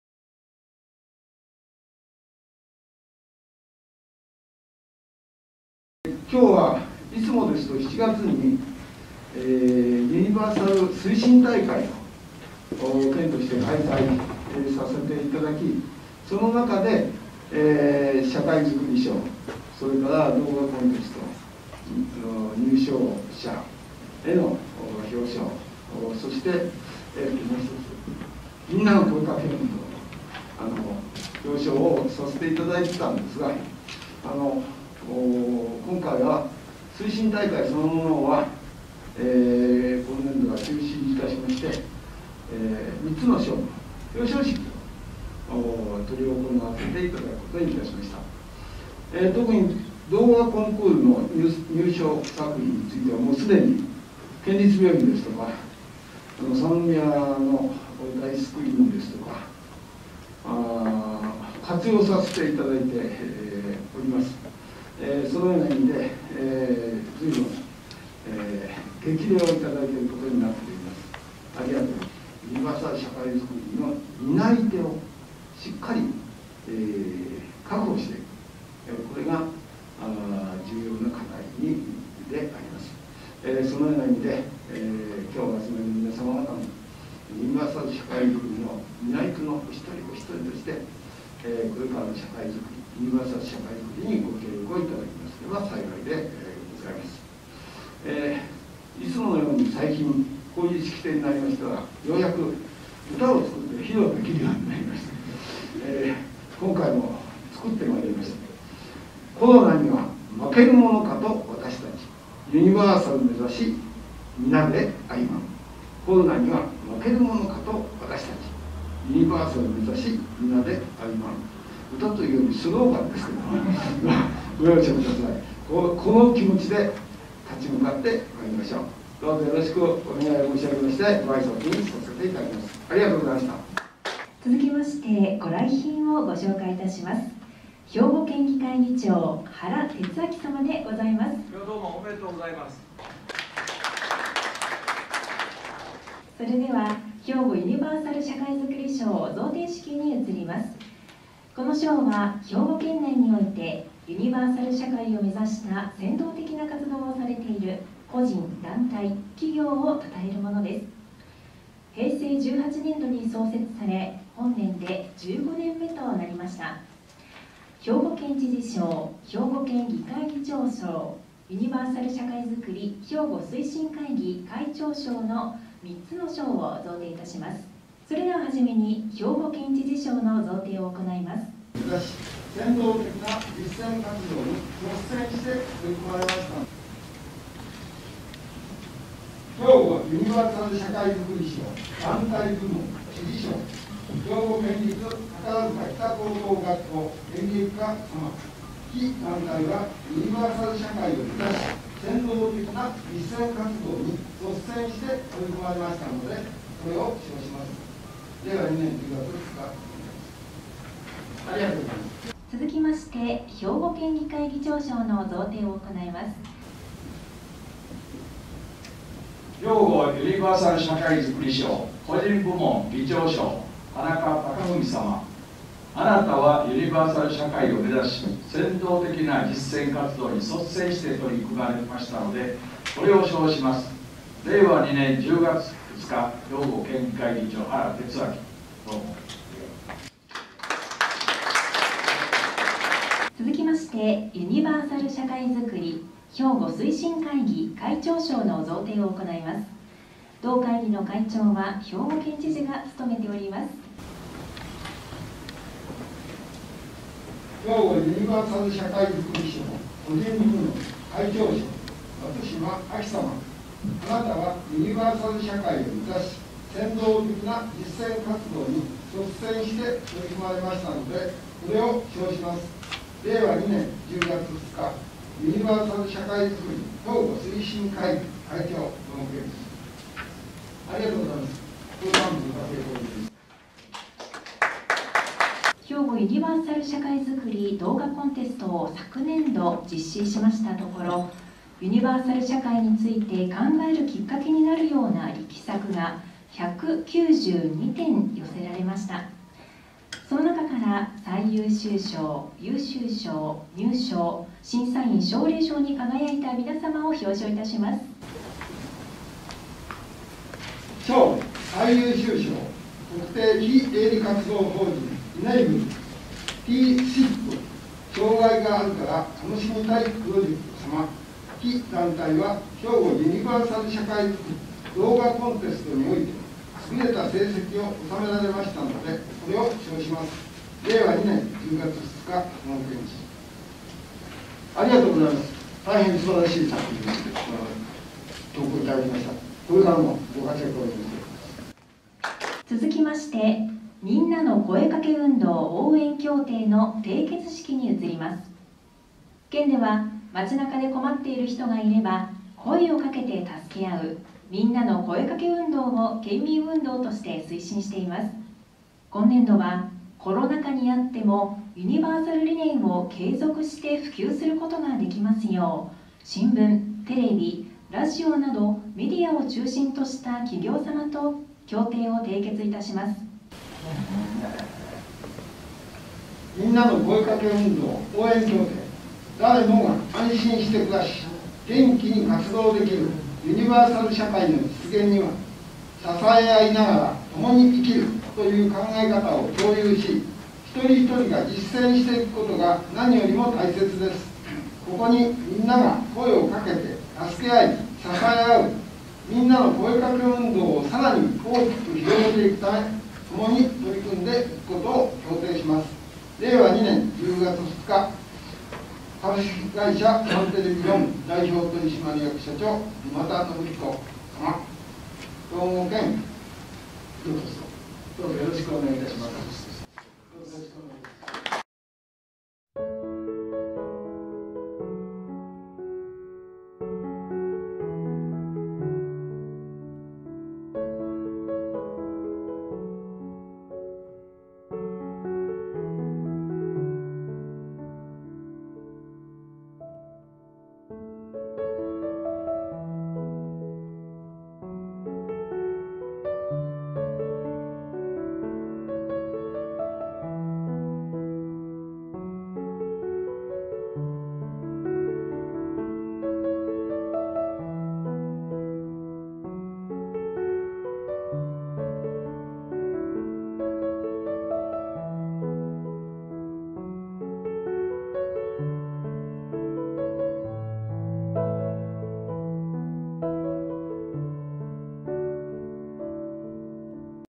令和2年度ひょうごユニバーサル社会づくり賞贈呈式、ひょうごユニバーサル社会づくり動画コンテスト表彰式、みんなの声かけ運動応援協定締結式（ダイジェスト）
令和2年10月2日に兵庫県公館で「令和2年度ひょうごユニバーサル社会づくり賞贈呈式、ひょうごユニバーサル社会づくり動画コンテスト表彰式、みんなの声かけ運動応援協定締結式」を開催しました。

○ひょうごユニバーサル社会づくり賞受賞者

○ひょうごユニバーサル社会づくり動画コンテスト受賞者

○みんなの声かけ運動応援協定締結団体